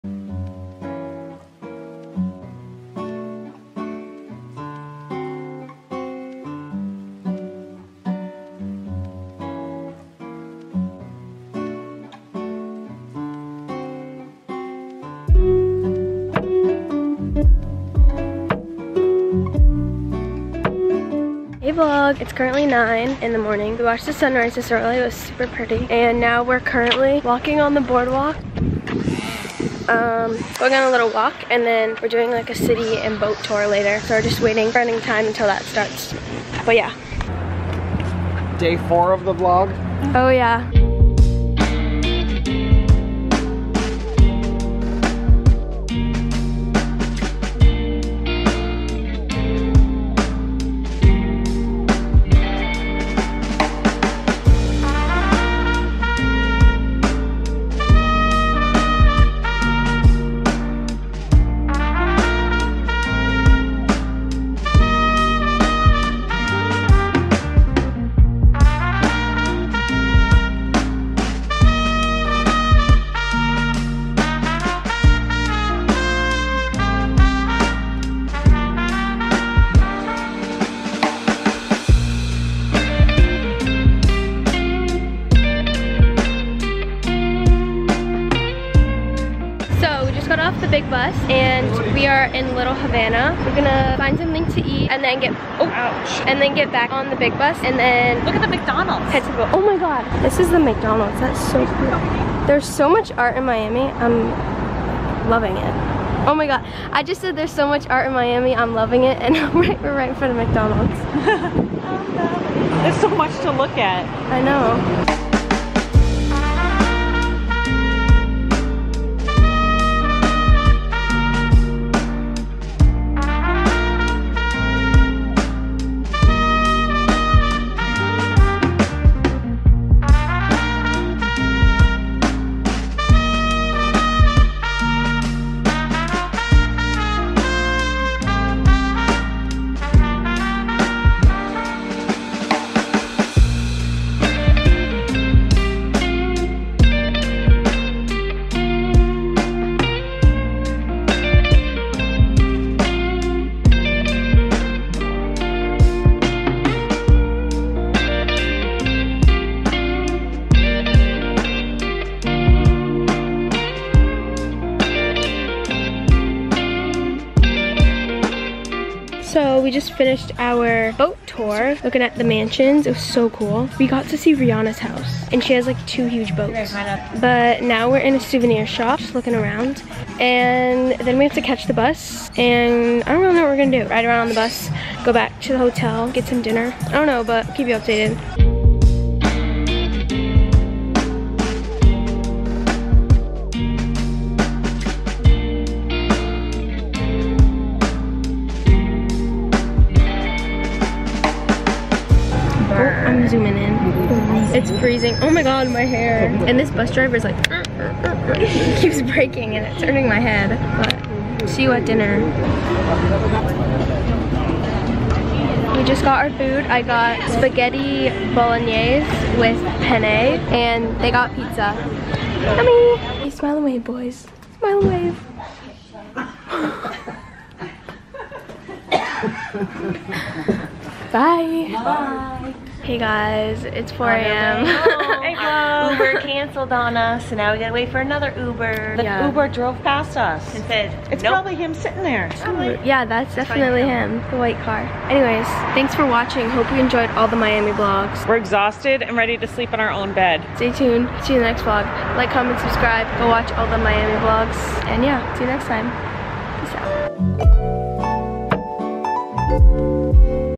VLOG: miami episode 5 (big bus tour & exploring)
here is the fifth video (and final vlog) from our miami trip!

this was our last day and we decided to have a chill, exploring day. we went on the big bus tour and the millionaires row boat tour.